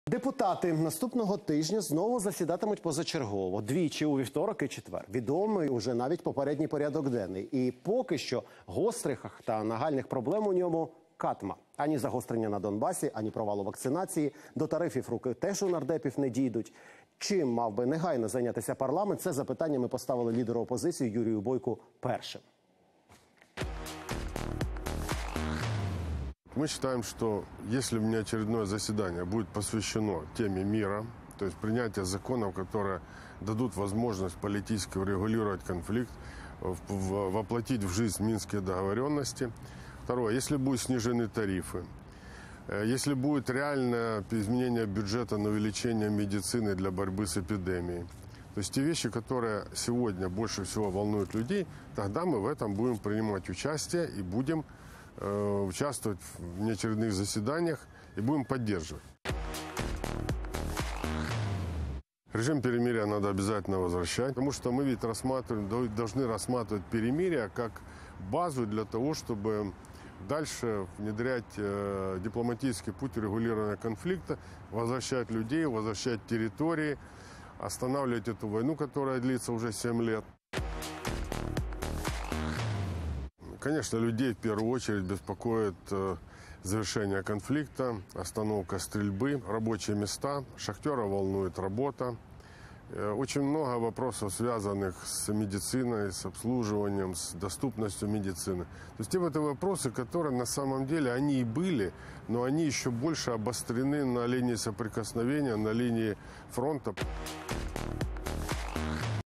"Бюджет, тарифи, перемир'я": Юрій Бойко оприлюднив головні пріоритети для Парламенту
Депутати наступного тижня знову засідатимуть позачергово - чим мав би негайно зайнятися Парламент? Це запитання ми поставили лідеру опозиції Юрію Бойку